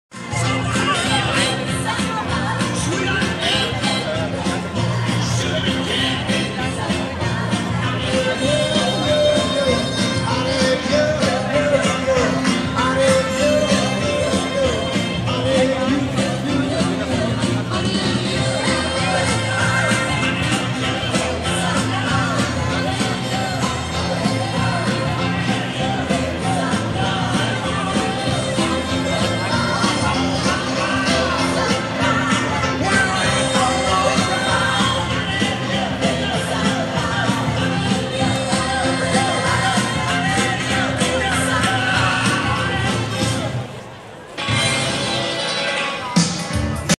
The Frankfurt Half Marathon March 12th 2017
Outside "The Commerzbank Arena" near the start line for The Frankfurt Half Marathon 2017